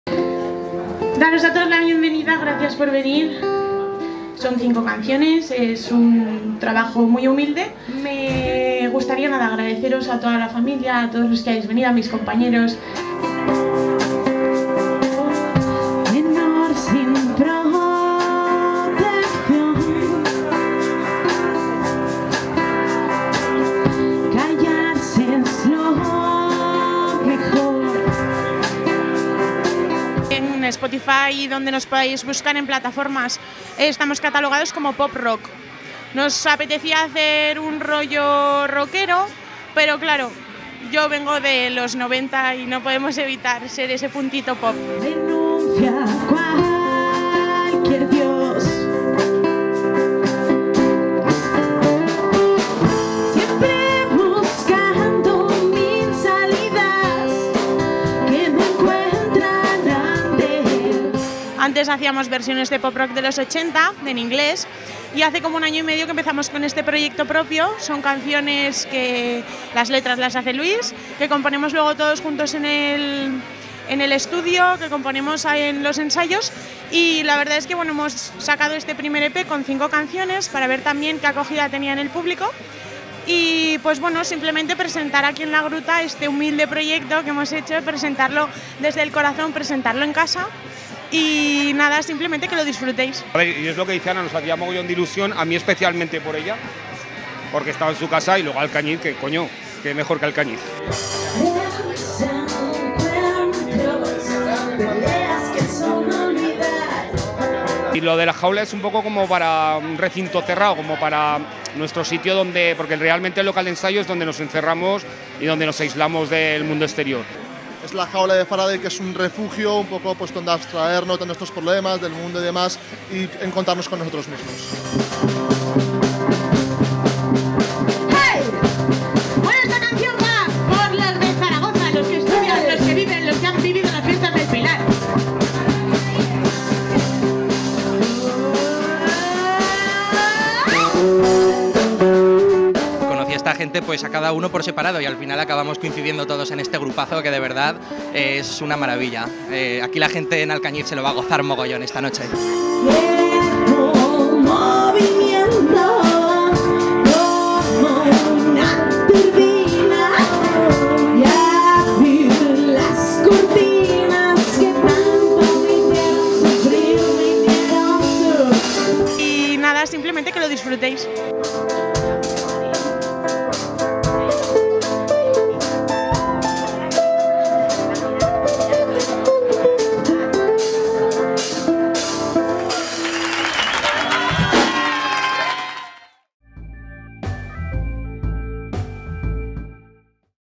La Jaula de Faraday presenta su nuevo disco
La gruta ha acogido la presentación del nuevo disco de la Jaula de Faraday,  un disco que cuenta con 5 canciones aunque solo tocaron un remix de 10 minutos durante la presentación. ¡Os deseamos muchos éxitos!